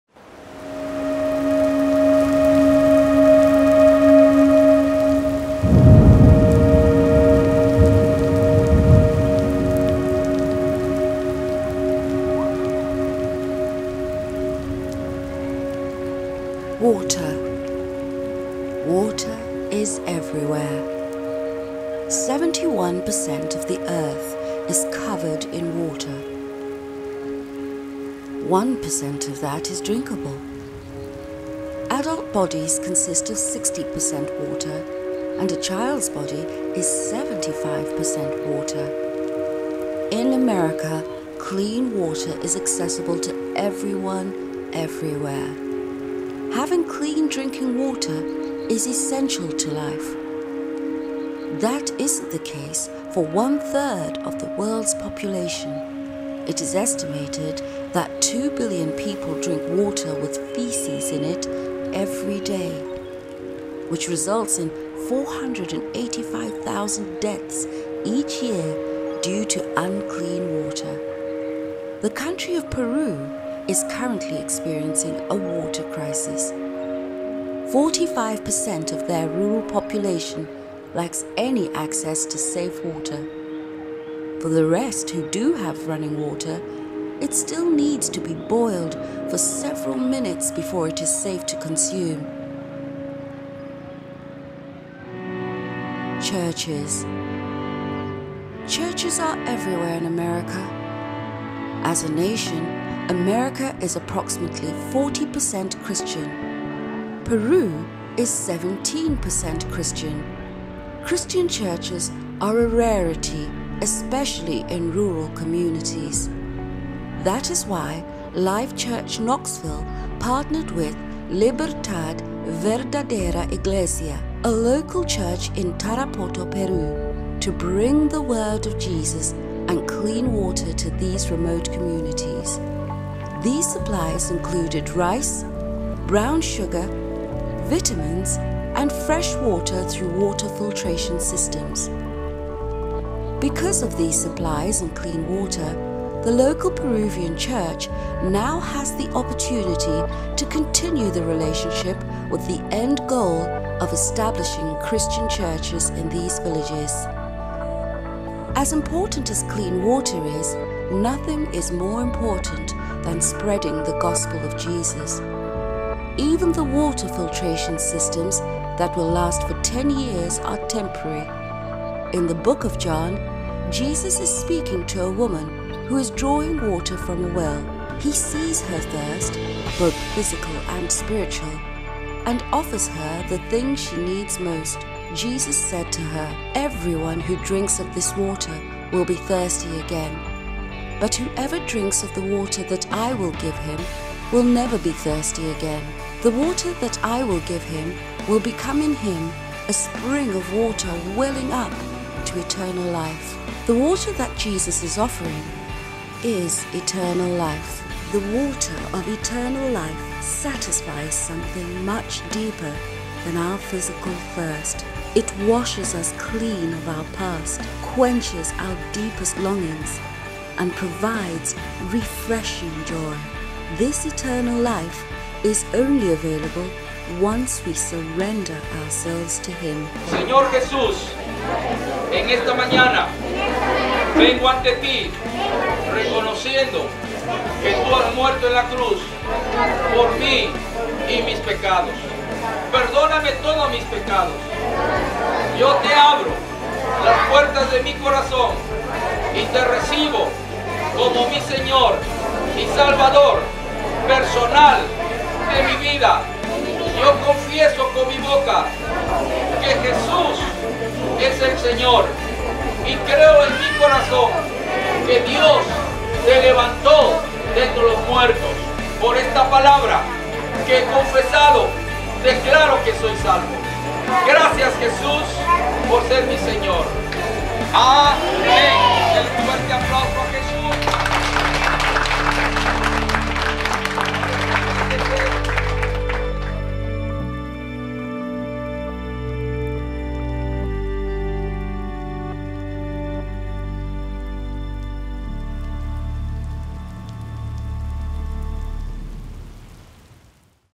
Water of Life - Short Documentary (2020)
Clean water is essential to life, but not everyone has access to it. Two churches, one from Knoxville, TN and one from Tarapoto, Peru, teamed up to bring clean drinking water to rural Peruvian communities and spread the Gospel of Jesus. Subtitles are available in English and Spanish.

El agua limpia es esencial para la vida, pero no todo el mundo tiene acceso a ella. Dos iglesias, una de Knoxville, TN y otra de Tarapoto, Perú, se unieron para llevar agua potable limpia a las comunidades rurales peruanas y difundir el Evangelio de Jesús. Los subtítulos están disponibles en inglés y español.

Jana Vaughn Salley
Jhonatan Sam Carvallo
Adan Valdemar Carvallo Diaz
Pilar Gonzales Pizarro
Lauren Blevins
Robert Wayne Salley
Melanie Nordstrom
Jenny Castleberry
Rhonda K. Cowell
Johnathan Schutz
Timothy Schutz
Zach Fowler
Morgan Willer